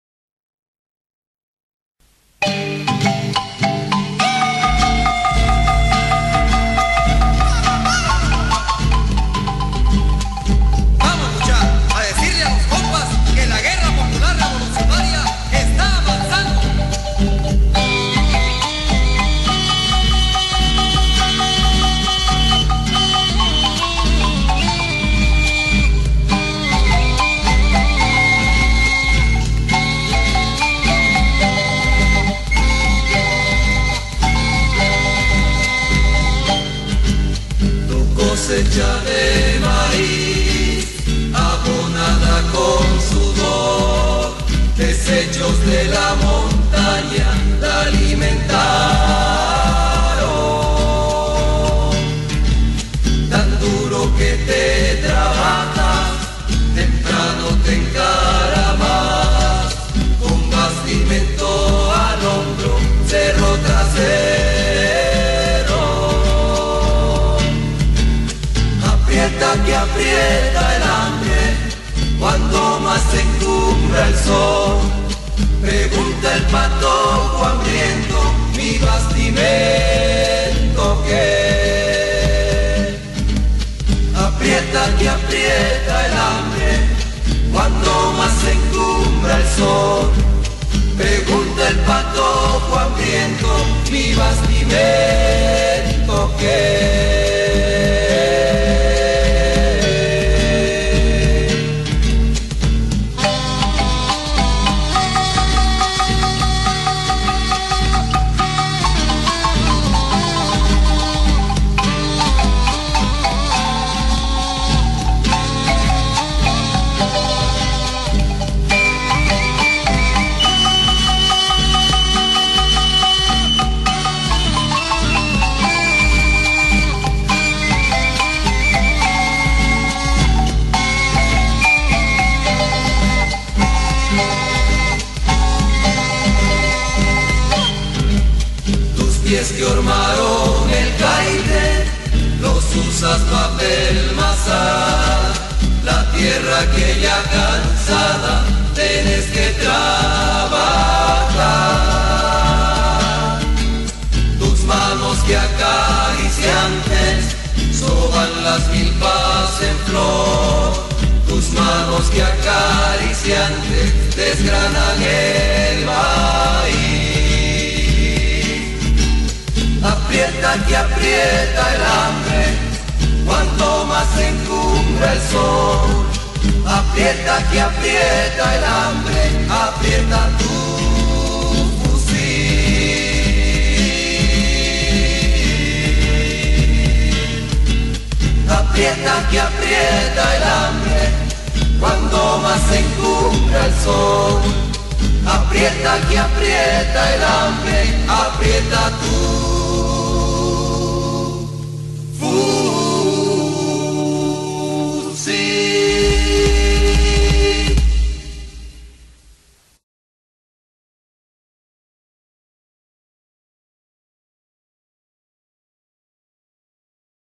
taller de municipalismo de URNG
se realizó el taller de municipalismo en Tiquisate Escuintla, se contó con la participación de organizaciones sociales y sindicales del area, con una participación de casi 250 compañeras y compañeros.